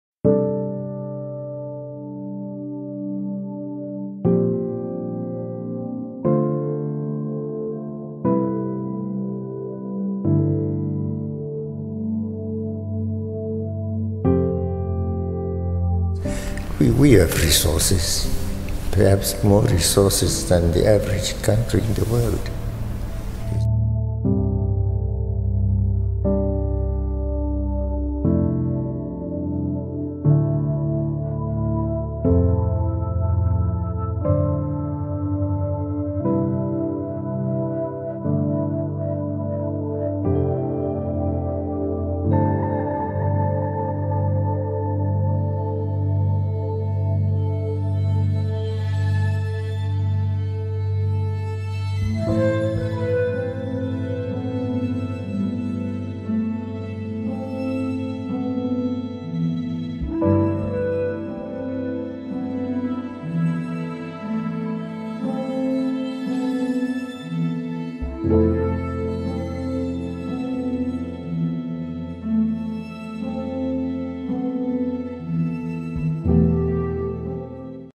Who Is Robert Mugabe? | Los Angeles Times
Robert Mugabe, the longtime president of Zimbabwe, says the liberation of Ghana in 1957 inspired his political awakening, according to BBC News.